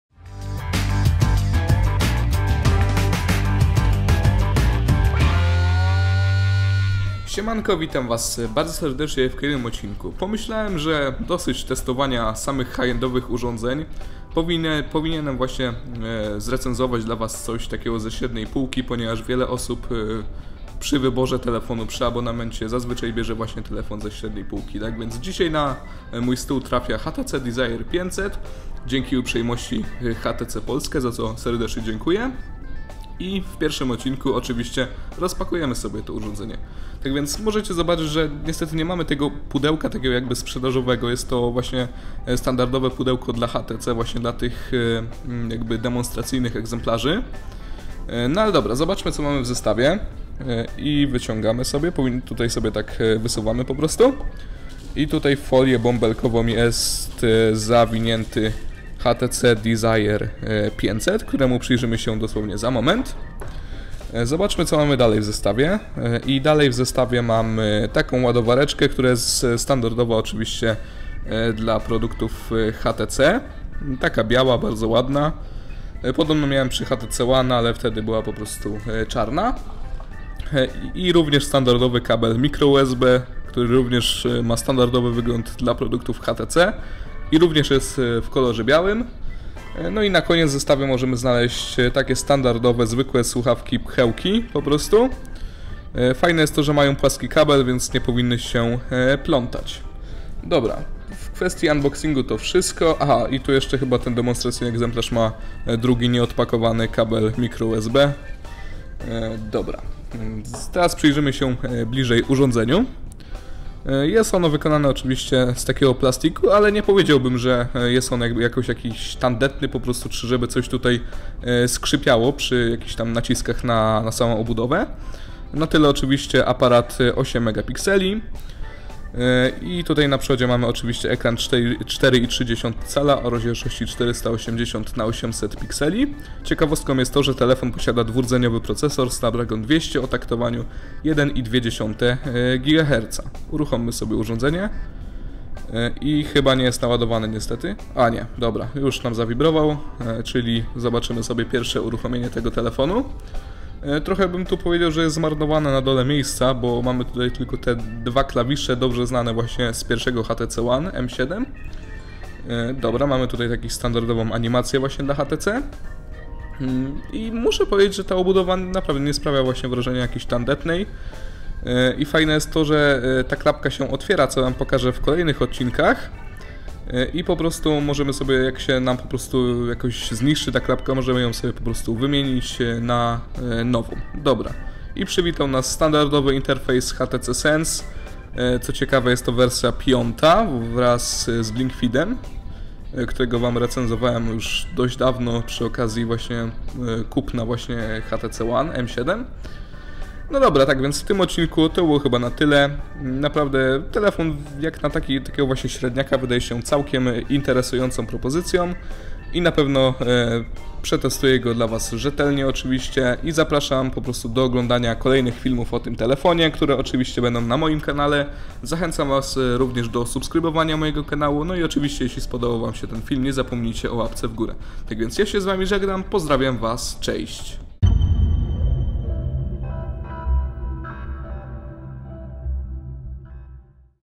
HTC Desire 500 - Rozpakowanie - Unboxing (PL)
Tym razem rozpakuję dla Was smartfona ze średniej półki, czyli HTC Desire 500. Zapraszam do oglądania!

W razie jakichkolwiek pytań lub problemów piszcie na maila, który znajdziecie poniżej :-)